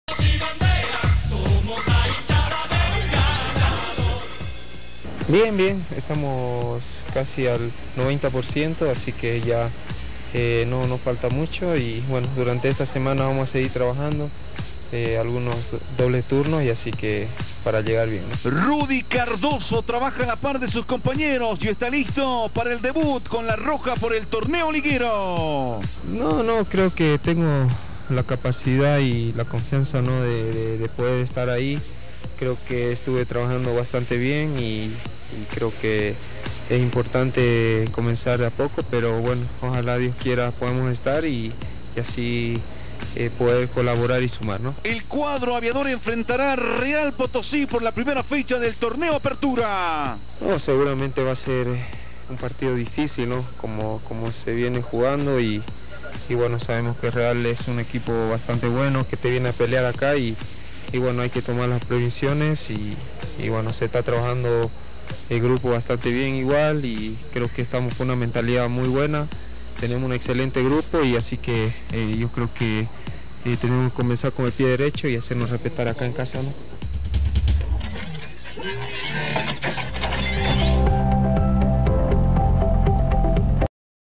Recuerdo de 2017: Rudy Cardozo habla de sus objetivos en Wilstermann
Rudy Cardozo a su llegada a Wilstermann prometia hacer historia con el Club cochabambino